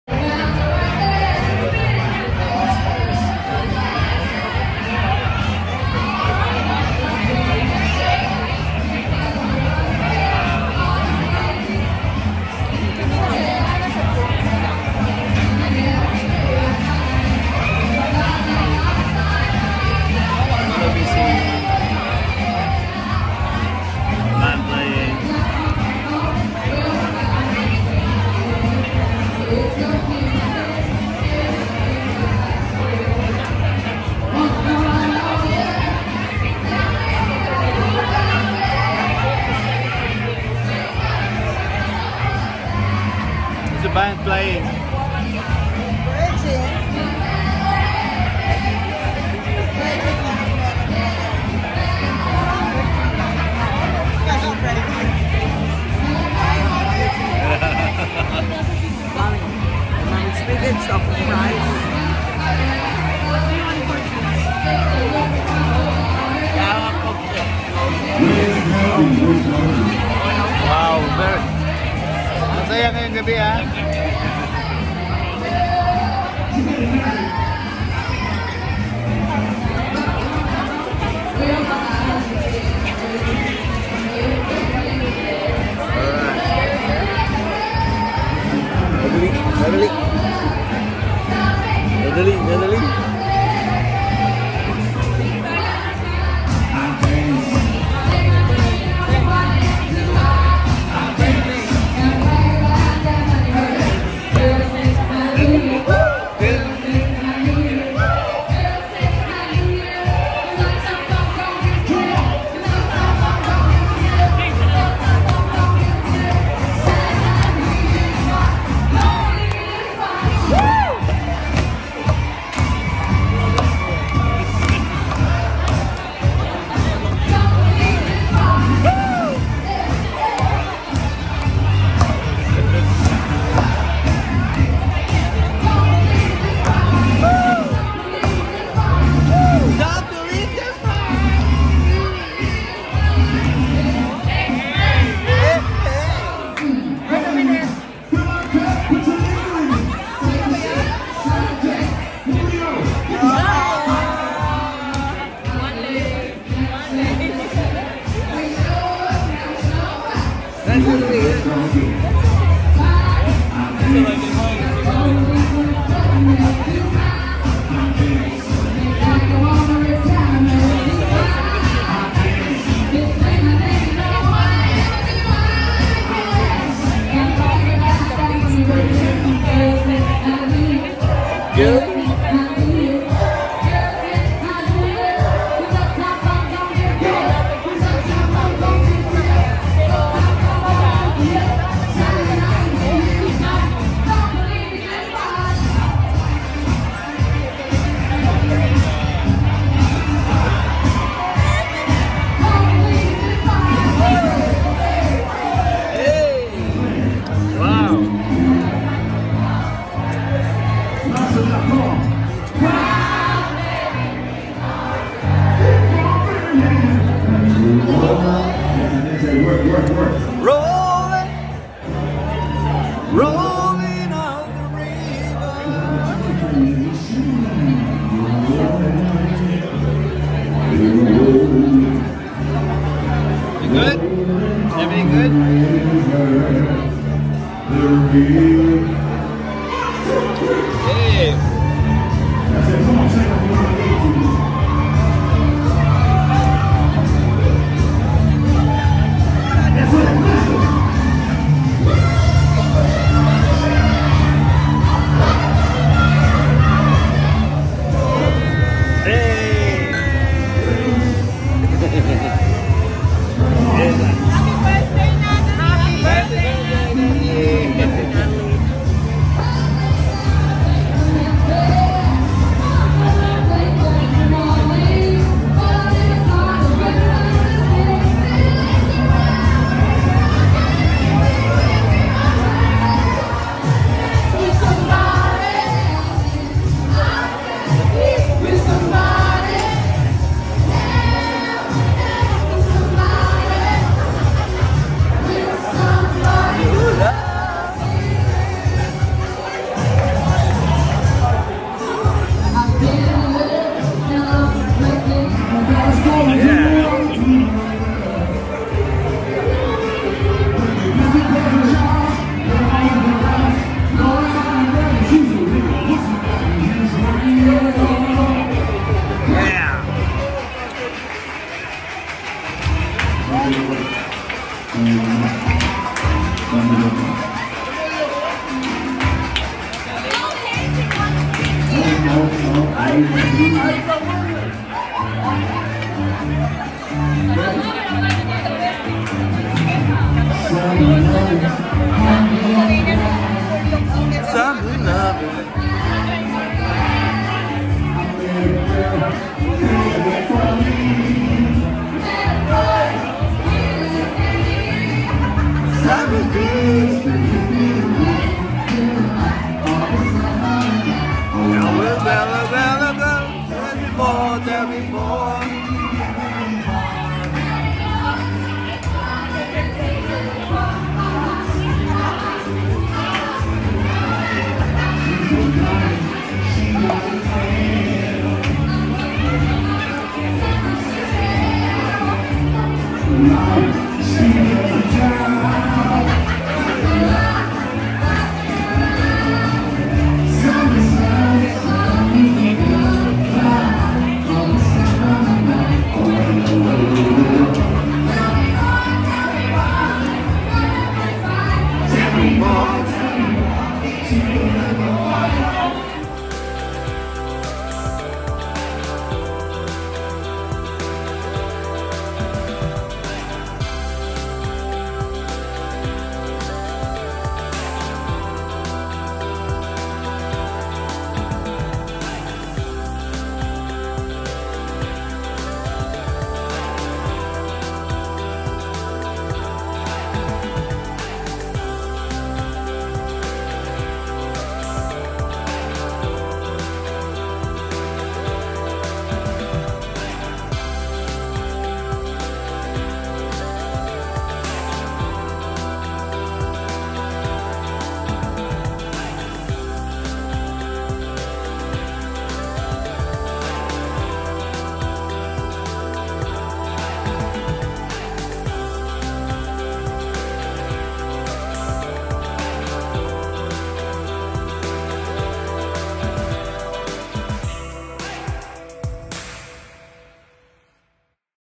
White Rhino Bar and Restaurant
Dinner at White Rhino Bar and Restaurant, Surfers Paradise, Gold Coast, Queensland. Beautiful food and listening to a live band. It was a great night along with friends.